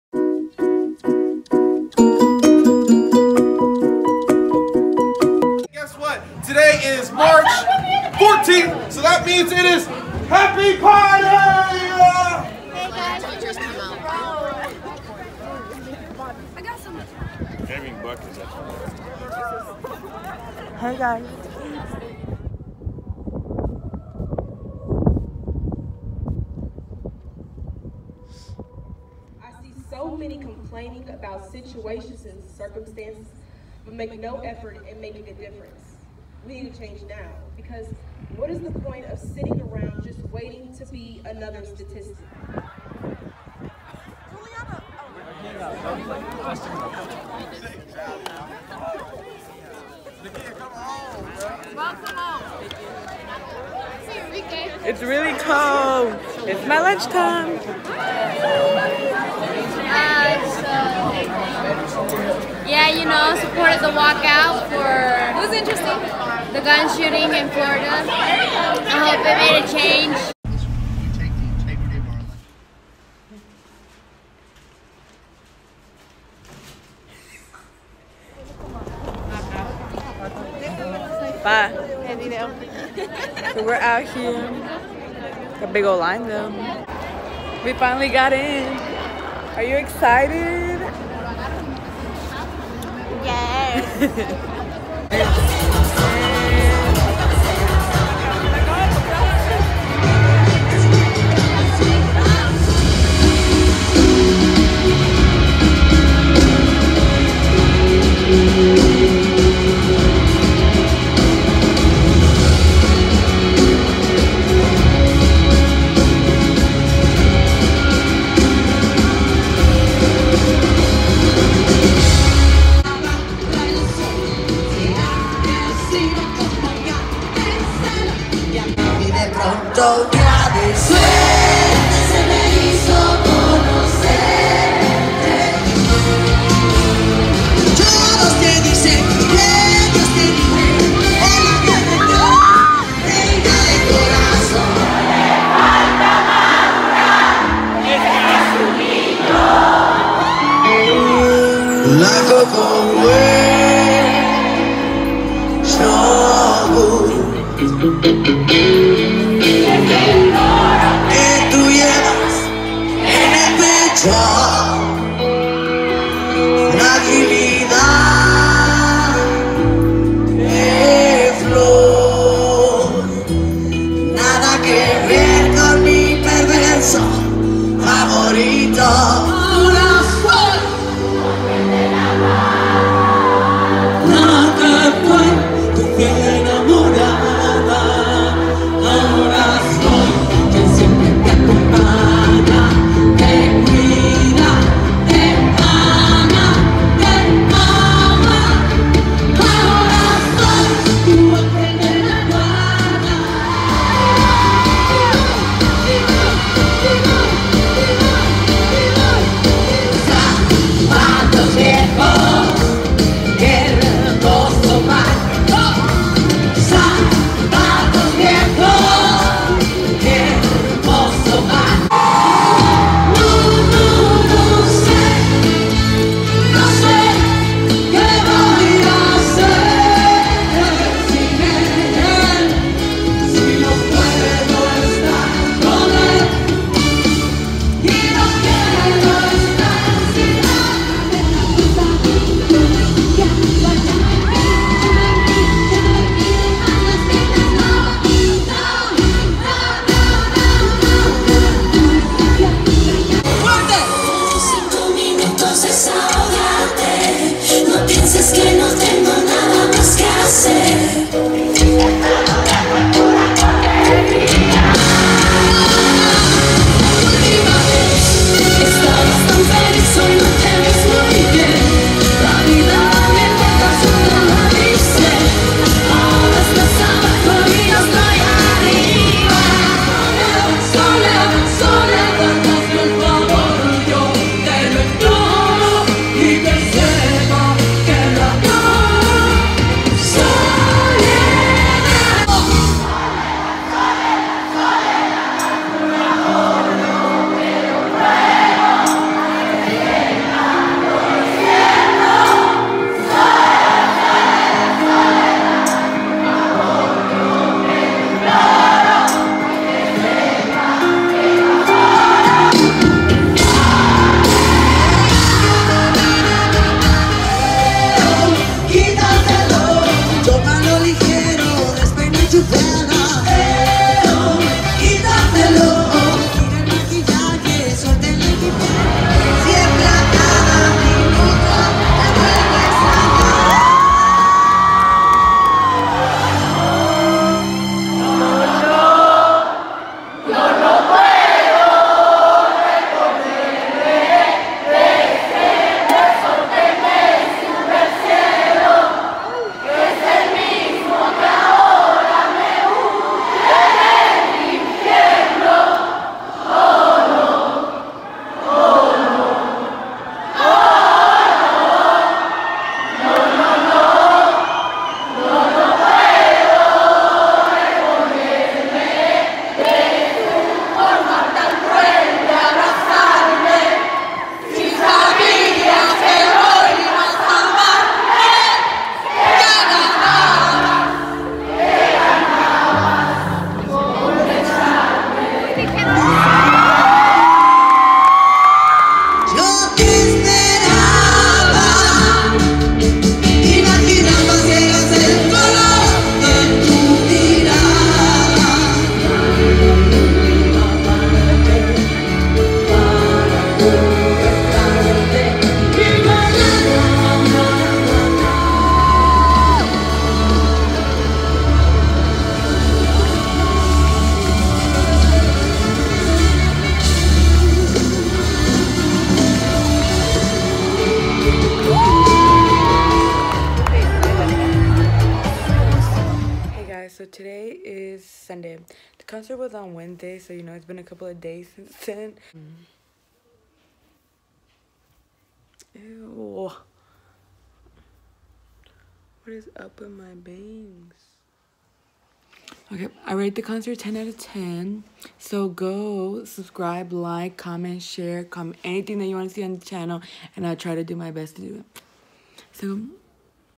2018 school walkout & Alejandra Guzman vs. Gloria Trevi concert Vlog #2 | Jullss
Sorry guys if it’s a little bit blurry :(
This is how March 14th of 2018 went ! It was pretty exciting XD
I had a lot of fun.
We got to participate in National School Walkout Day to show that we support the kids in Florida that are trying to make a change.
I also got to go to the Alejandra Guzman vs. Gloria Trevi concert and it was LIT !